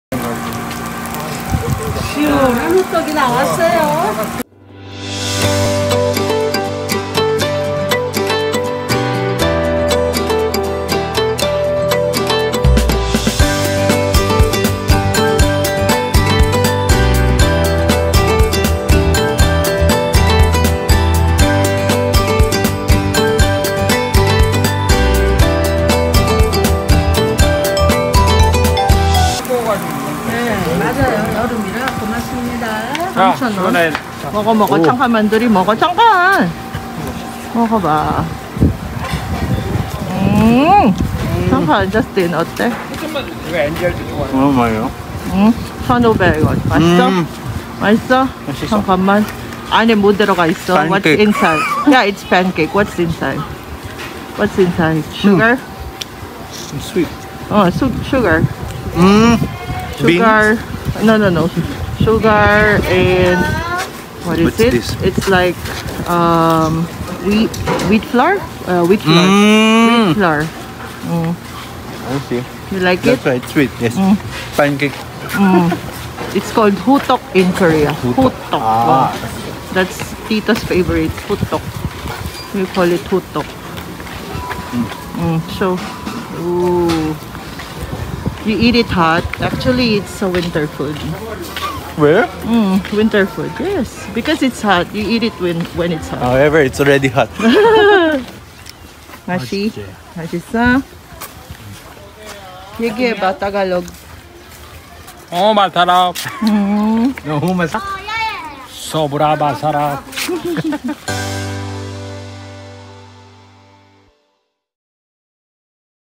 HOTTEOK MUKBANG (호떡 먹방)
Annyeong chingudeul.

I would like to introduce another famous STREET FOOD IN KOREA.
It is called HOTTEOK (호떡).
It is a type of Korean pancake made of dough from wheat flour, water, milk, sugar, and yeast. Inside Is  filled with a sweet mixture, which may contain brown sugar, honey, chopped peanuts, and cinnamon.
We went to the beach and there is a store selling it.

This is my husband’s favorite snack and Justin tried this for the first time.
So they did the HOTTEOK MUKBANG (호떡 먹방) together.

To know more about this, please like and watch this video.